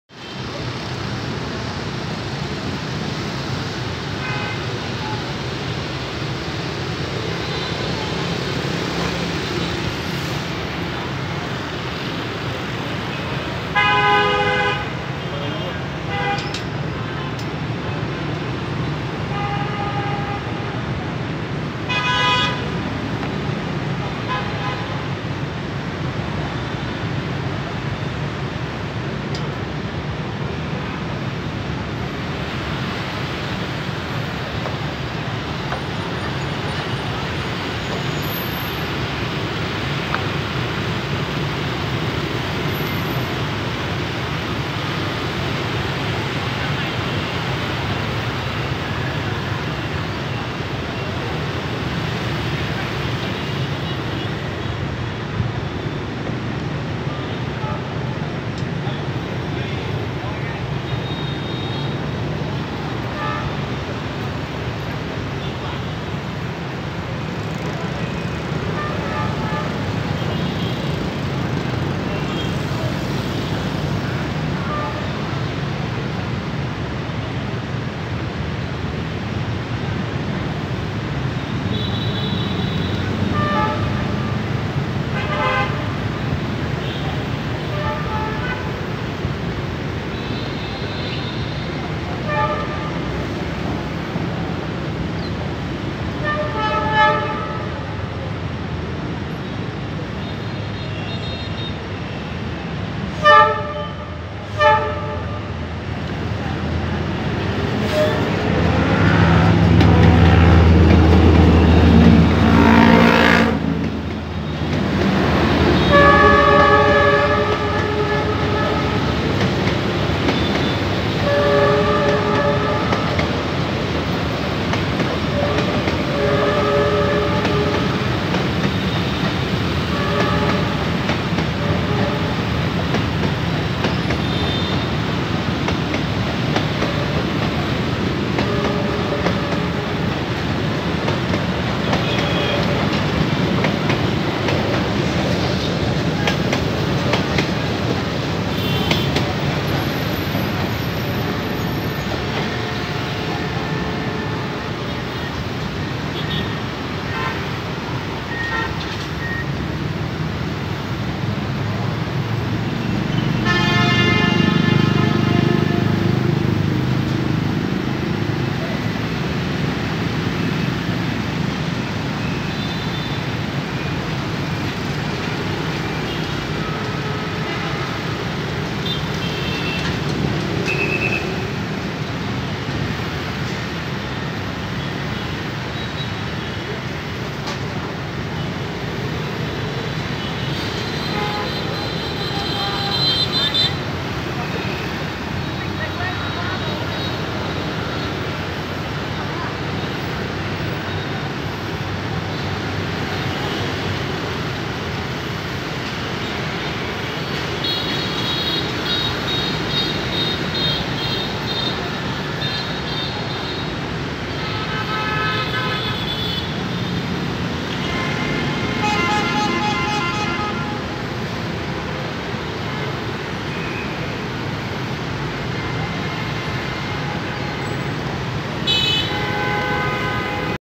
D19E-945 pulls train SE24 through Bach Mai barrier to Hanoi station(3/2/2024)!11:06 am!
I saw a train behind me so I turned around and the second train came running!